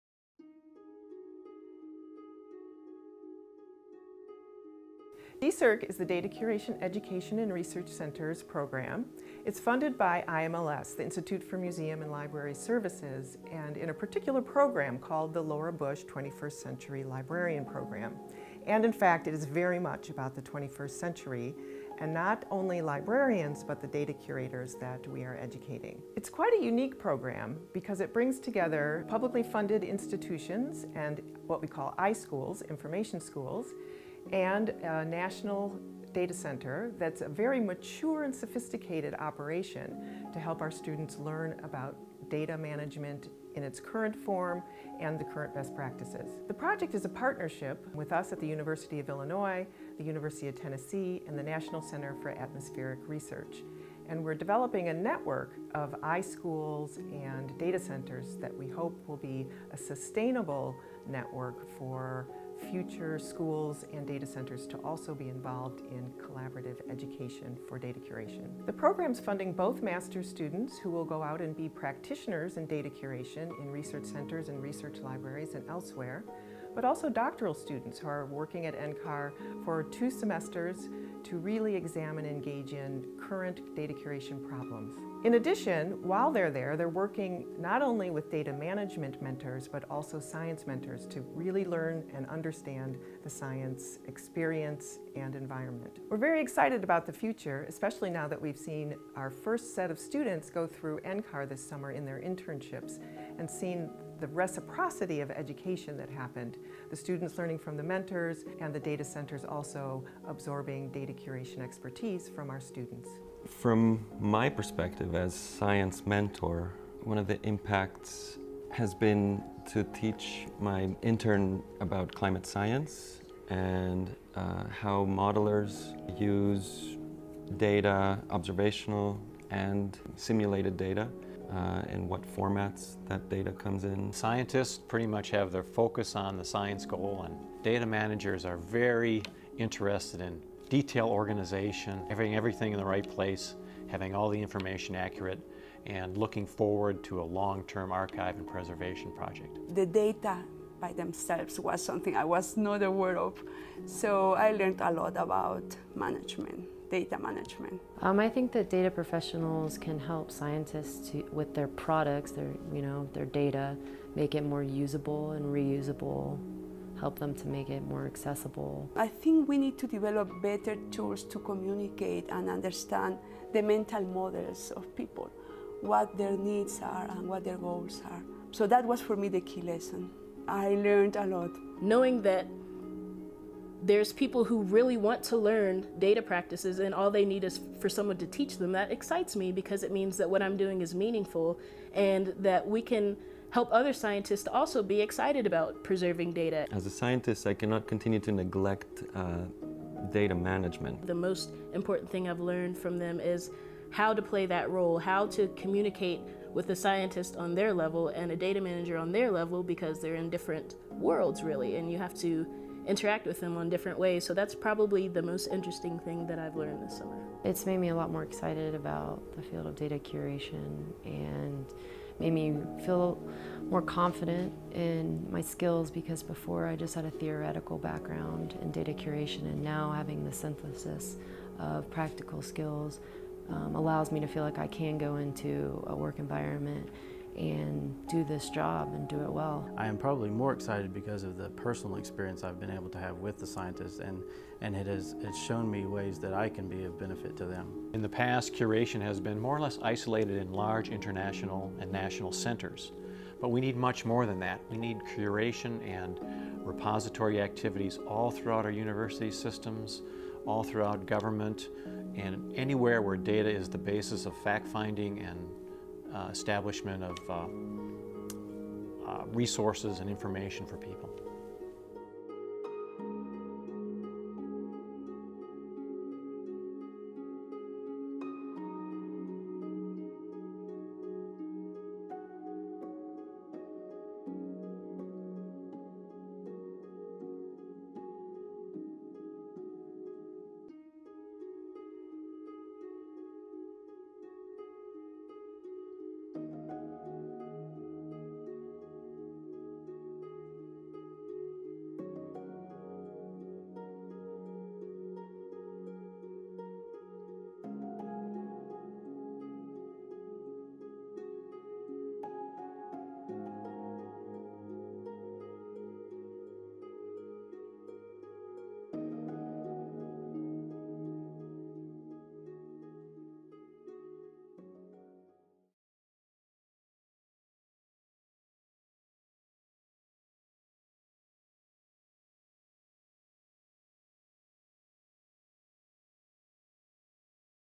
DCERC proves to be a successful model for data curation education
The Data Curation Education in Research Centers program (DCERC) is a model for data curation education, coupling high-quality curriculum with field experiences in research and data centers. This unique program educates library and information science students at the master's and doctoral-levels, and builds on the strengths of the University of Illinois at Urbana-Champaign Graduate School of Library and Information Science and the University of Tennessee at Knoxville School of Information Science The two iSchools have partnered with the National Center for Atmospheric Research a world-renowned data center, to make it possible for students to learn state-of-the-art data management and curation practices. DCERC is funded by a grant from the Institute for Museum and Library Services (IMLS; as part of the Laura Bush 21st Century Librarian Program.

During the program, students are immersed in the real world of scientific data curation, engaging in current techniques and challenges. They work with science mentors as well as data mentors to fully understand the context of data production and use in the scientific research process. The outcomes for the first cohort of master's students has been highly successful: experiences have aligned with student career goals, every student completed projects that lead to conference posters or papers, and all evaluation measures have been positive. The summer internships resulted in a valuable exchange—students learned from mentors and the mentors also learned from the students. We are truly excited about the future of this program.